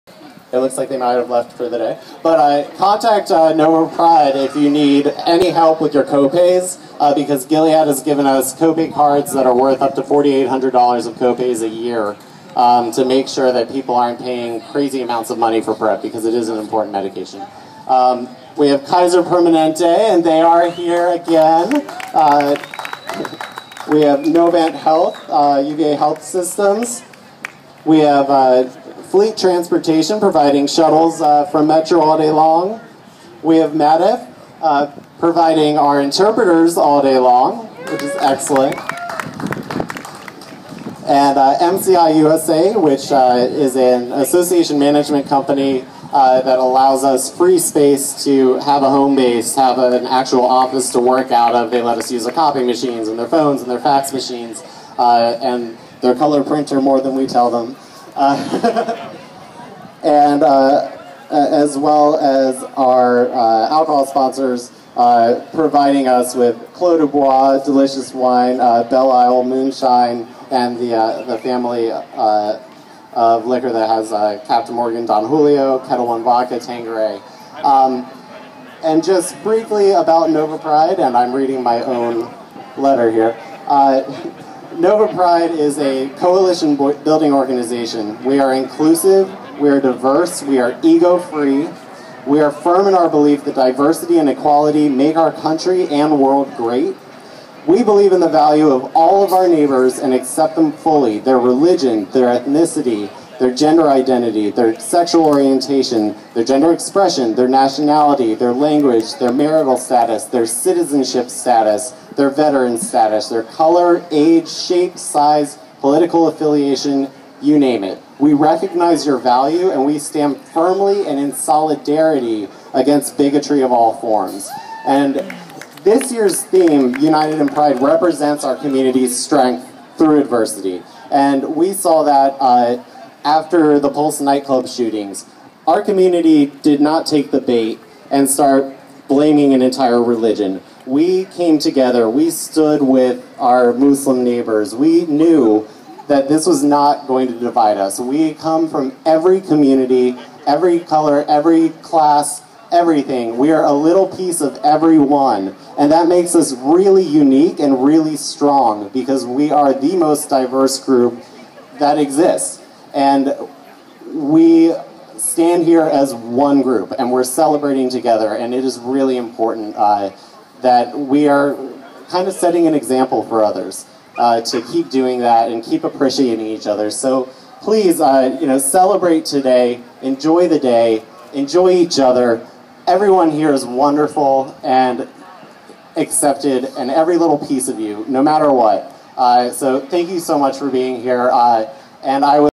Brian Reach @ NOVA Pride 2018 (Part 2)
Here's a video clip of Brian Reach speaking at NOVA Pride on September 29, 2018.